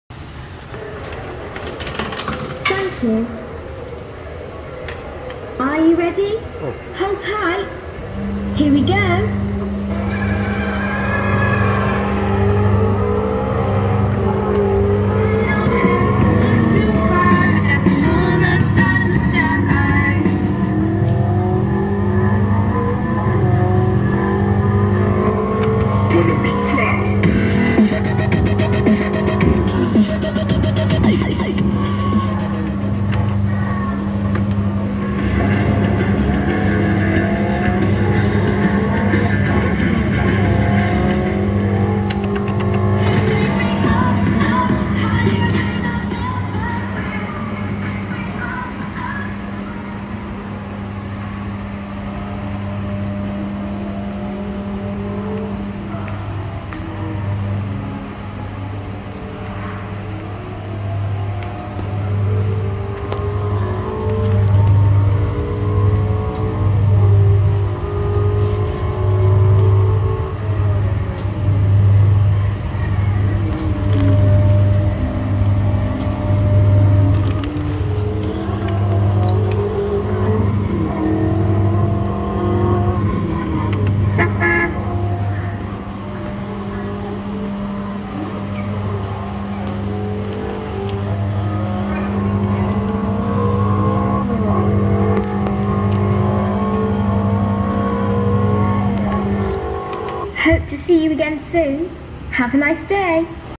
Northern Leisure MK2 Rally Car Coin Operated Kiddie Ride
Filmed At The Coventry Transport Muesum In Coventry In Saturday 9 December 2023

Made, Manufactured And Released By Northern Leisure Group Limited / Kiddy Ride's UK In Leed's In 2018, Under No License Released In 2019 As Well Suchs as  MK2 Police And MK2 Rally Car

This Ride Is the Newest Latest Kiddie Rides By Northern Leisure Group/Kiddy Rides UK

This Ride Is Based on Some Rally Car and Also This Ride 6 Button's In Total 1 Start Button 2 Blue Interactive Sound Horn Button's
And 4 Musics Button's Also Features Colorful Lights For Newest Rides

This Kiddie Ride Features 2 Speaker

This Kiddie Ride Features 2 Steering Wheelz

This Kiddie Ride Plays a Car Drive Races Racing Driving Sound Effects Plays Drift at the Start at the Ride

This Particular Model is Red

This Particular Model is Owned By Northern Leisure Group Limited / Kiddy Ride's UK (As Well it's also Sold By Northern Leisure Group Limited / Kiddy Ride's UK, As if it has the Kiddy Ride's UK Logo,) And Kidzone

Northern Leisure Group Limited Code:
Serial Number: MR 5608
Model: MK2 Rally Car
Voltage: 240Vac 50Hz